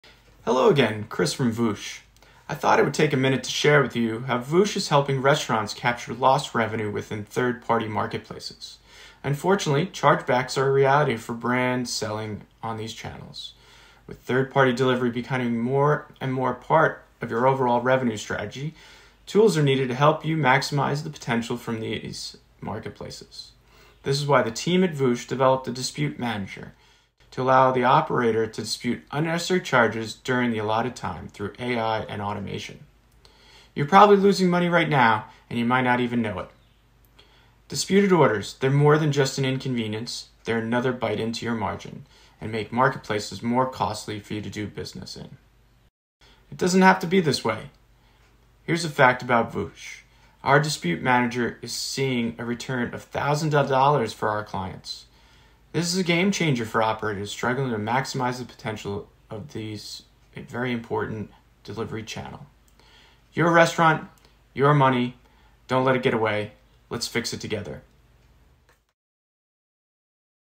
Stop losing money unnoticed! Start your free trial now
🍕 Dispute Manager by VOOSH: Maximizing Restaurant Revenue in the Age of Food Delivery Apps 🚗
Welcome to our deep dive into VOOSH's innovative solution for restaurants: The Dispute Manager. As third-party food delivery platforms like DoorDash, UberEats, and GrubHub dominate the landscape, it's essential for eateries to capture every dollar they rightfully deserve.

Are you a restaurant owner or operator? Ever noticed discrepancies in your earnings? You're not alone. Chargebacks are becoming a common issue for brands selling on these platforms, and sometimes, you're losing money without even realizing it.

💡 Introducing VOOSH's Dispute Manager
VOOSH's AI-powered Dispute Manager is a game changer. Instead of letting unnecessary charges erode your margins, this tool aids in disputing unjust charges swiftly and efficiently. Gone are the days of manual checks and lengthy resolution processes. Let technology do the heavy lifting.

📈 Real Results
The fact is, our Dispute Manager has already returned thousands of dollars to our clients. This tool isn't just about cutting losses; it's about enhancing your profit margin and making the most of platforms like DoorDash, UberEats, and GrubHub.

🤝 Let's Level the Playing Field
Disputes and chargebacks are more than just inconveniences. They directly impact your bottom line. As the food delivery ecosystem grows, it's time to ensure you're not just a participant but a key player who gets their due. Together, let's make sure you're not leaving money on the table.

🔗 Get Started with VOOSH's Dispute Manager Today
Ready to reclaim your lost revenue? Start your free trial now